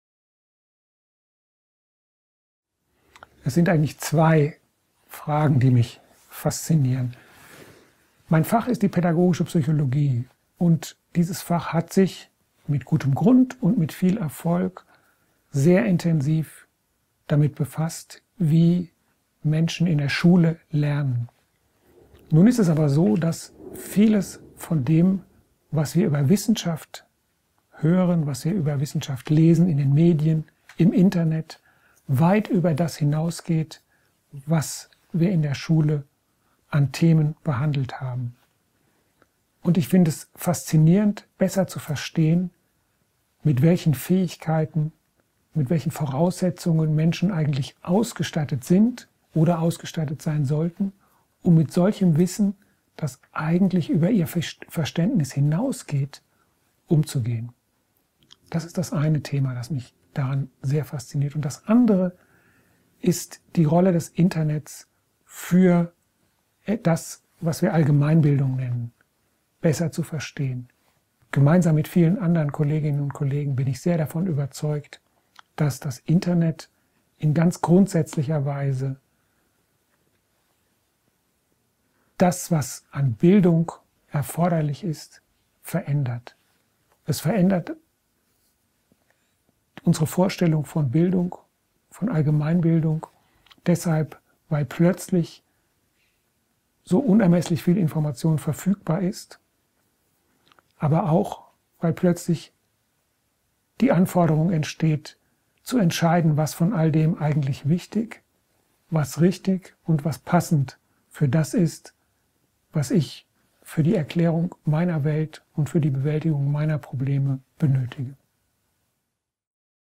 Richtig oder falsch? Prof. Dr. Rainer Bromme; PD Dr. Marc Stadtler - Die Faszination
Ob Stammzellforschung, Gen-Mais oder Grippeimpfung – im Internet können wir uns zu kontroversen Themen informieren. Doch oft stoßen wir dort auch auf widersprüchliche Antworten. Wie sollen wir entscheiden, was richtig ist, und uns eine Meinung bilden, wenn wir kein oder nur wenig Vorwissen zu einem Thema haben?